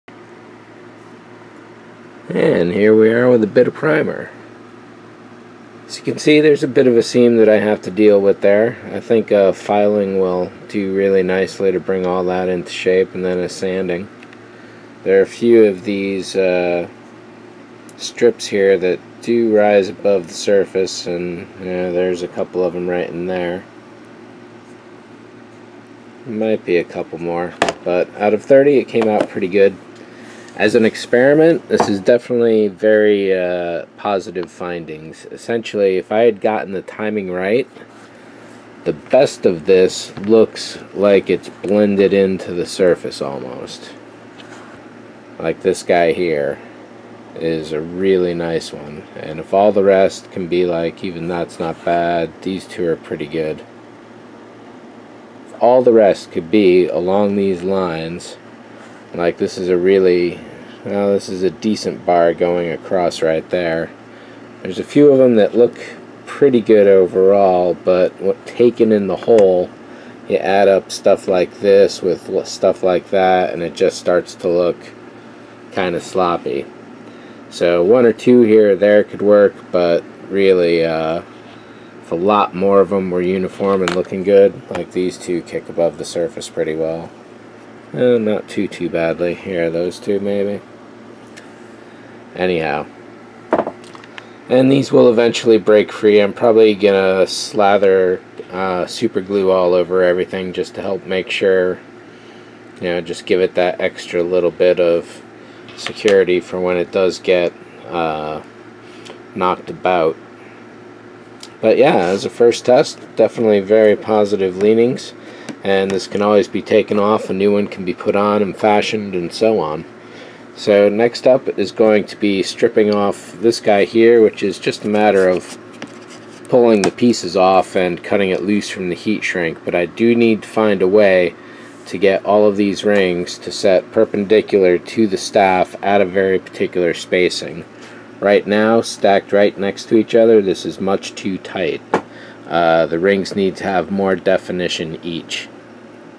VibroAxe 11D: Bubbler Notes
The idea is to make this staff modular and transportable.  With a pvc tube outside and a wooden dowel inside, this makes things very easy.  The largest part should fit into a suitcase for luggage, therefore the largest parts conveniently top-out at 18".

This video finds the project well along and past a major hurdle, such as it was.